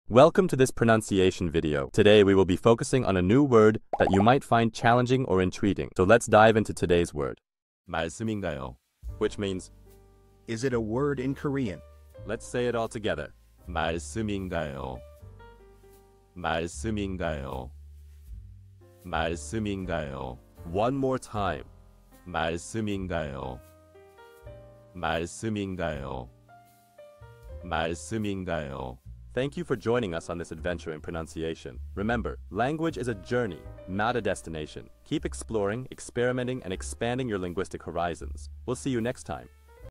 How to pronounce malsseumingayo | 말씀인가요 (Is it a word in Korean)
Master the Pronunciation of 'malsseumingayo | 말씀인가요' - which means : Is it a word in Korean 📝 with @PronunciationKorean 🎓🗣️ - [Your Guide to Korean]

Learn to pronounce 'malsseumingayo' perfectly in Korean with our easy and engaging tutorial by @PronunciationKorean! 📖📽️ In this comprehensive video, we provide you with native speaker examples 🗣️👂 and phonetic breakdowns 🎵 to ensure you get the pronunciation just right.